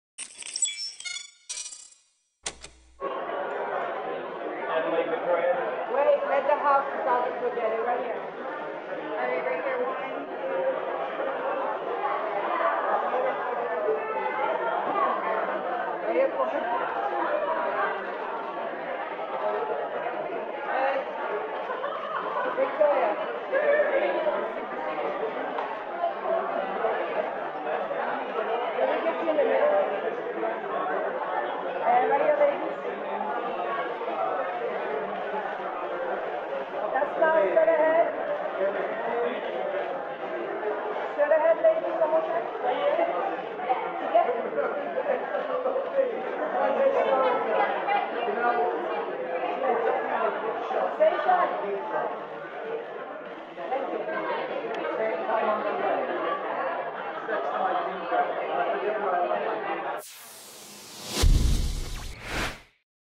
Analeigh Tipton, Victoria Justice 2019 Rom Com Fest “Summer Night” Screening Pink Carpet
Broll footage: Analeigh Tipton, Victoria Justice on the pink carpet at the Rom Com Fest screening of “Summer Night’ held at The Downtown Independent Theater in Los Angeles, California USA on June 21, 2019